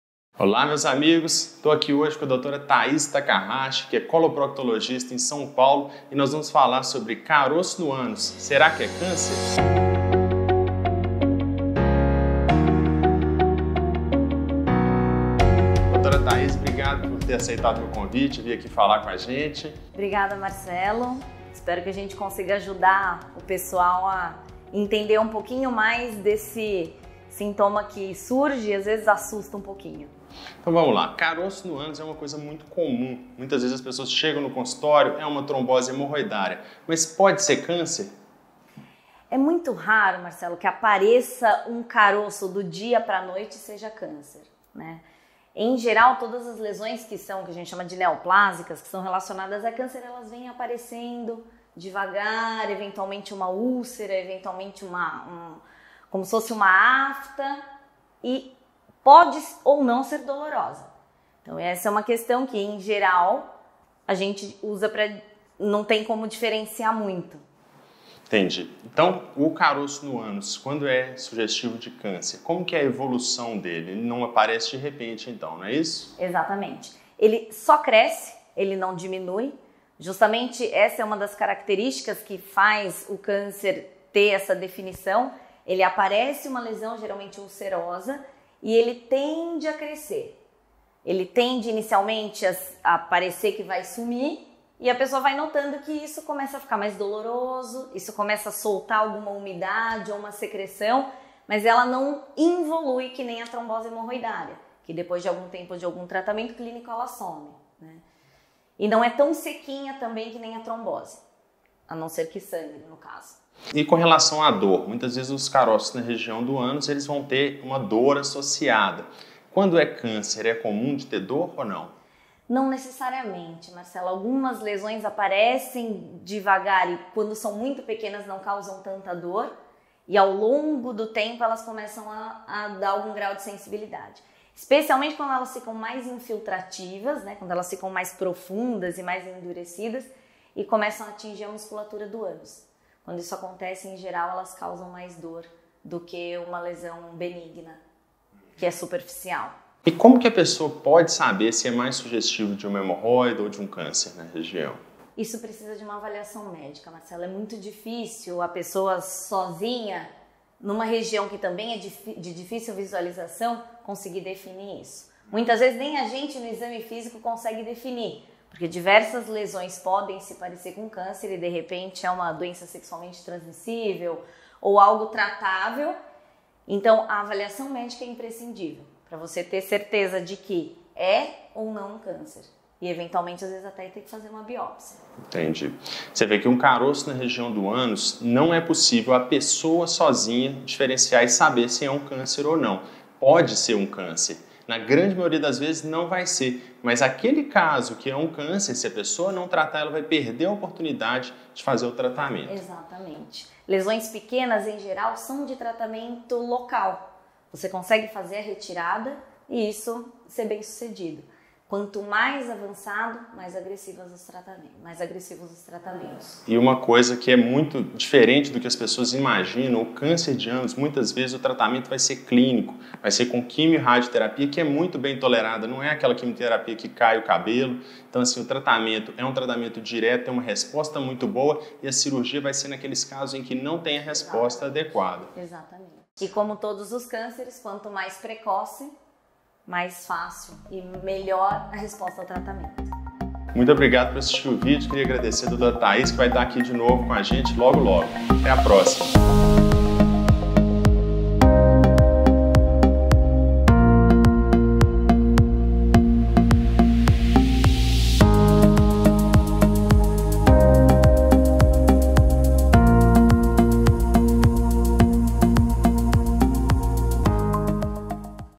Caroço no ânus, pode ser câncer?
Quando aparece um caroço no ânus, logo já imaginamos que se trata de um câncer! Mas será mesmo que o câncer nessa região aparece de repente? Vamos ver o que nos leva a pensar na possibilidade de um tumor ou considerar outras doenças, como hemorroidas!

VÍDEO EXCLUSIVAMENTE INFORMATIVO E DE FORMA ALGUMA SUBSTITUI UMA CONSULTA MÉDICA
Se ficar qualquer dúvida, mande nos comentários!

Rua Gonçalves Dias 142 sala 708 Funcionários BH/MG
Rua Bento de Andrade 146 Jardim Paulistano São Paulo-SP

Dr. Marcelo Giusti Werneck Côrtes
CRMMG 44832